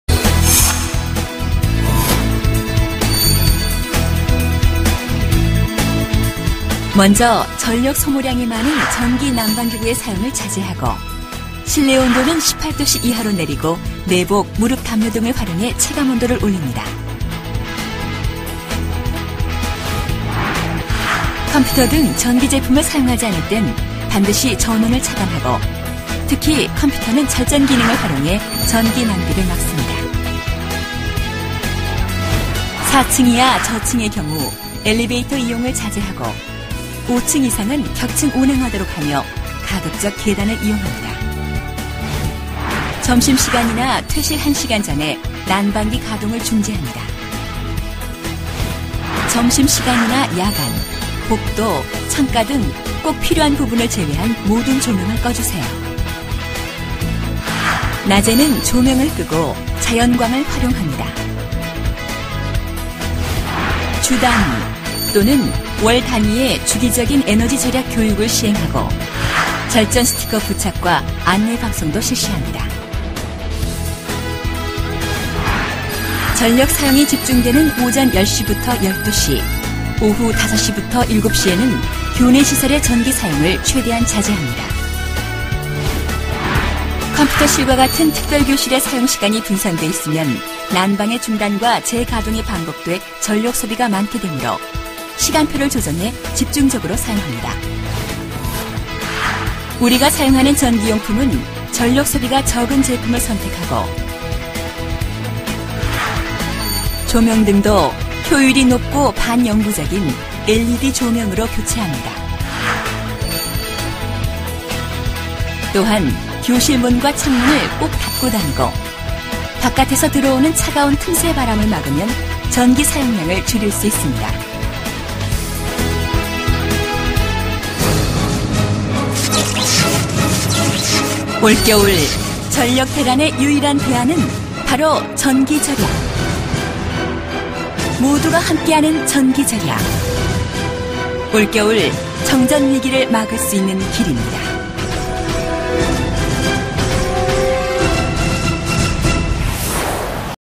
겨울철 전기절약 행동요령
에너지관리공단에서 전기절약행동요령을 알려드립니다.
전기절약, 어떻게 해야하는 지 잘모르겠다면 동영상을 보며 확인하시는 것도 좋은 방법이 될 것 같습니다.^^